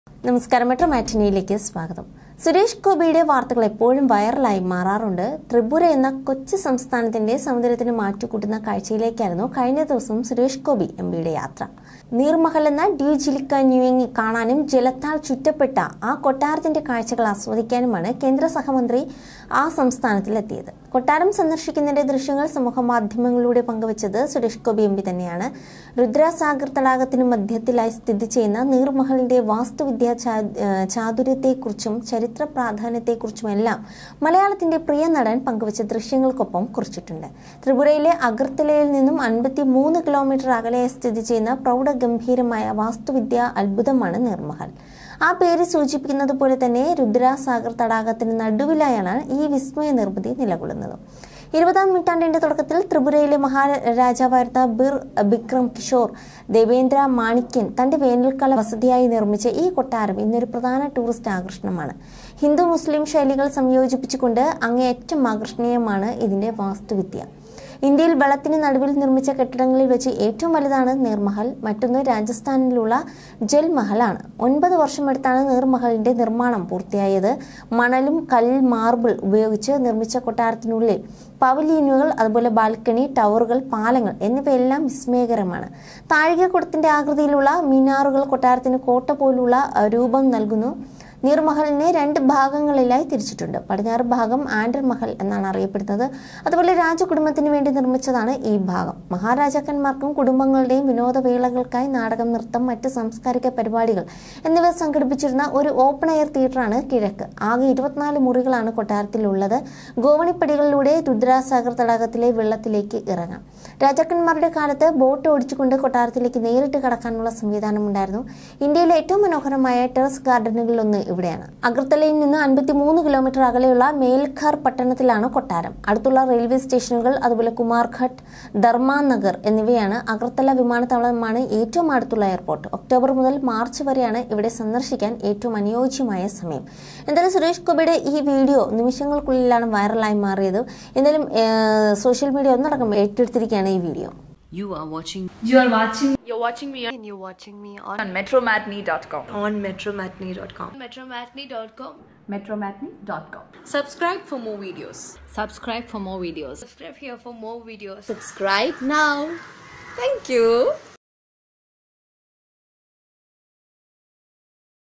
Sureshgopi | വൈറൽ വീഡിയോ ഏറ്റെടുത്ത് സോഷ്യൽമീഡിയ
പുറത്തിറങ്ങാൻ ആകാത്തവിധം
കൊട്ടാരത്തിനുള്ളിൽ പെട്ട് സുരേഷ്‌ഗോപി!
കരഞ്ഞു നിലവിളിച്ച് രാധിക.